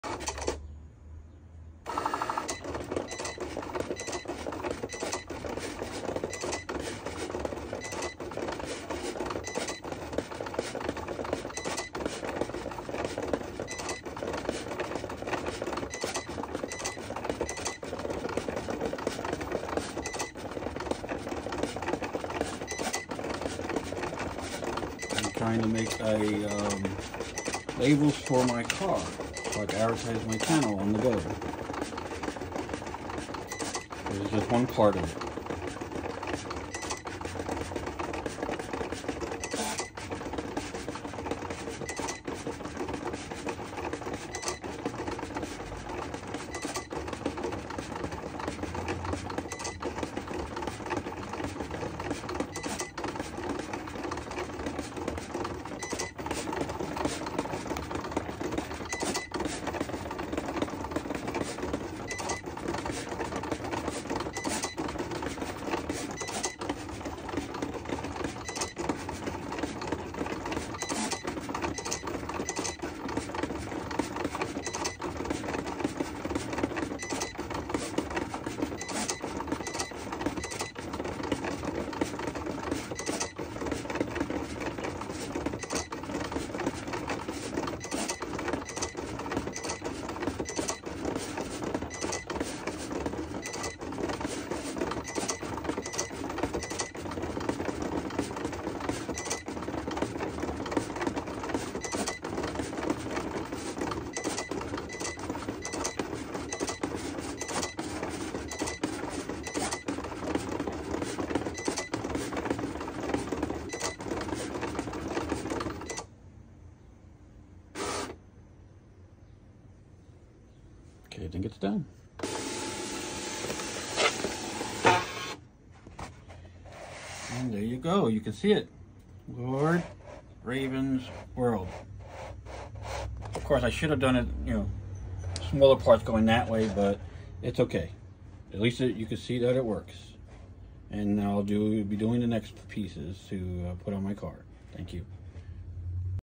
My channel label for my car
I’ll be doing advertisement for my car using my Cricut machine so I can still all the parts onto the back of my car for some free promoting. :)